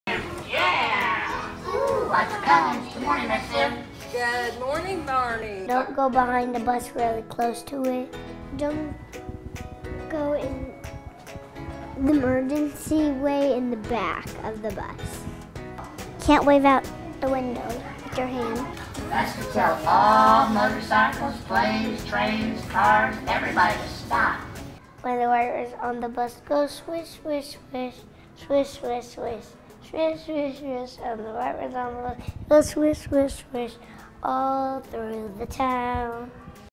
Barney the Bus Teaches ISD Students About Bus Safety
Barney the bus is making his way to all ISD elementary schools to teach students about bus safety! Listen to kindergarten students from Korte Elementary share what they learned about staying safe when riding the school bus.